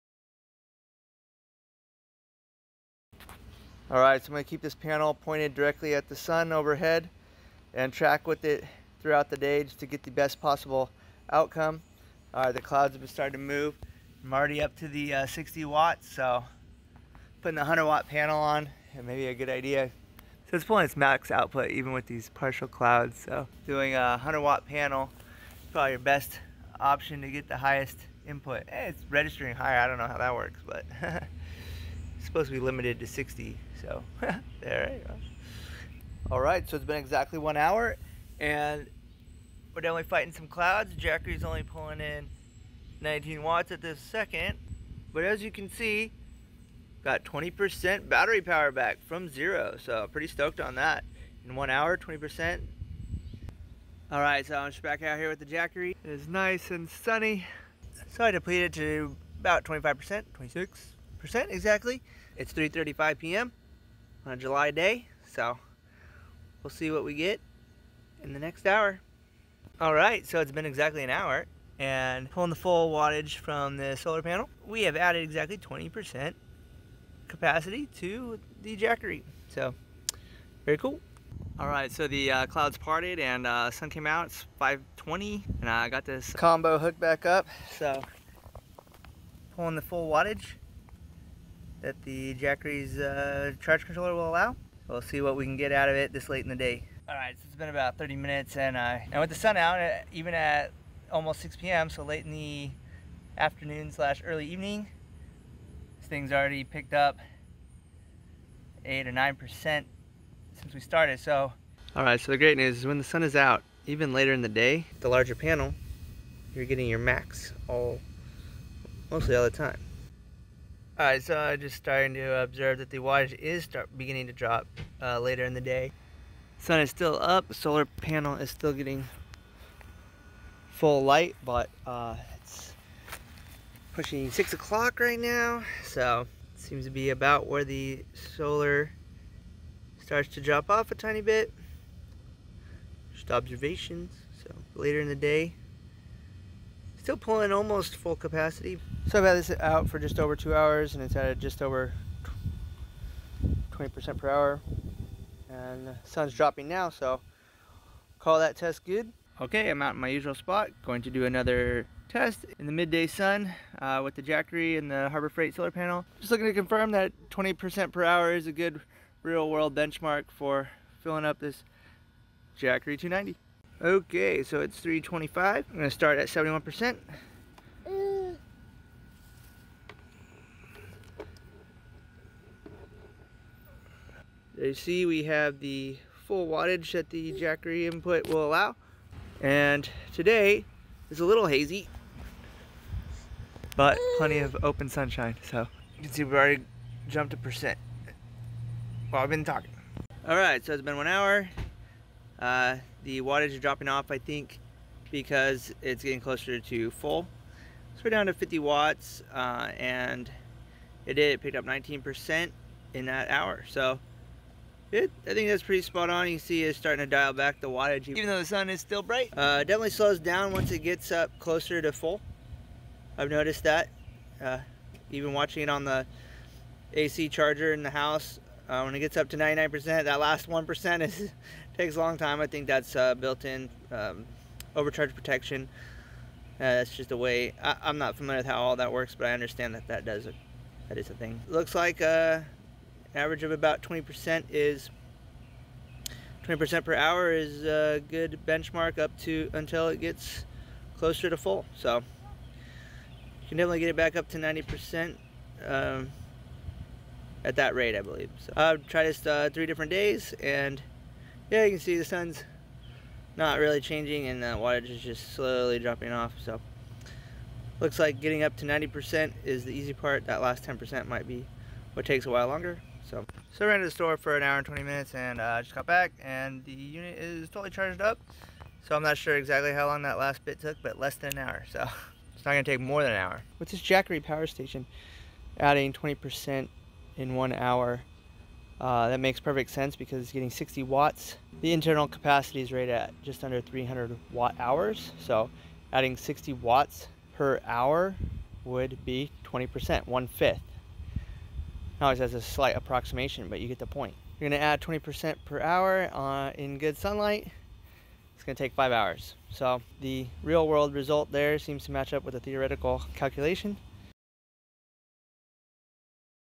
Jackery 290 + HF 100w panel - average sunny day charging
Its no problem to get the Jackery 290 charged up fairly quick with the Harbor Freight 100w panel - it recharges the Jackery battery with a 60w input which equates to about 20% per hour, at least up to the 90% mark when the Jackery starts dialing down the input wattage automatically. Very different outcome from our cloudy day results.